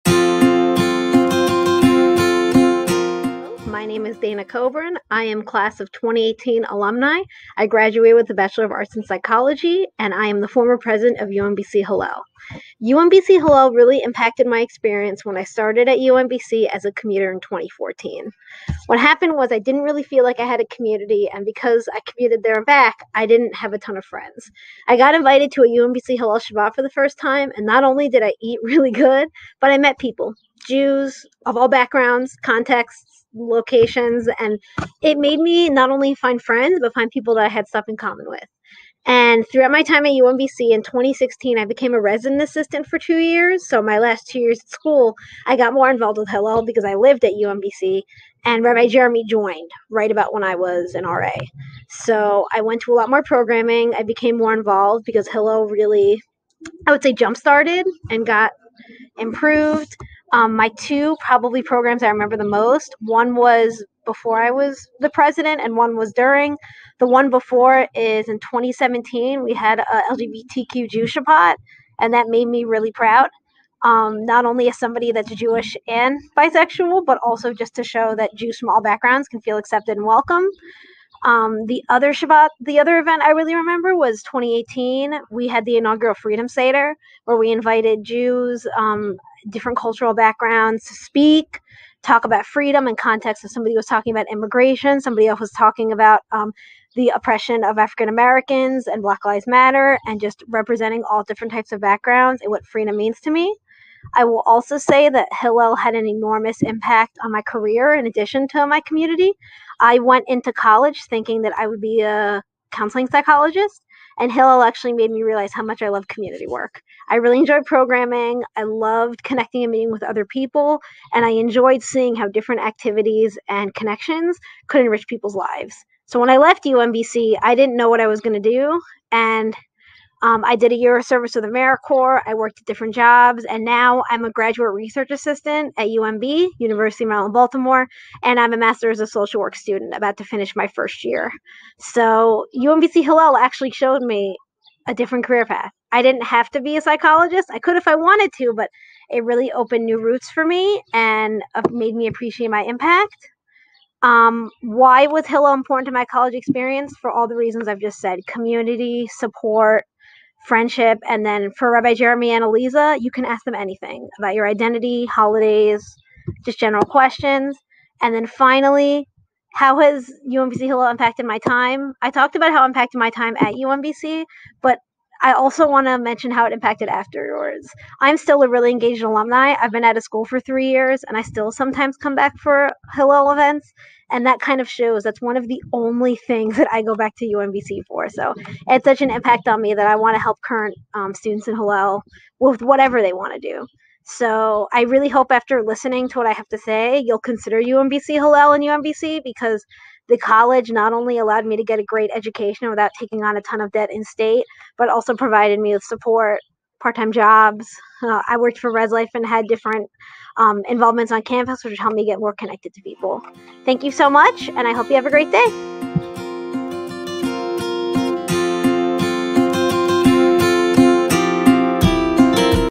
Alumni Testimonials: Dana Kobrin
What is the impact that UMBC Hillel has on students? Hear what Dana Kobrin, UMBC '18, has to say!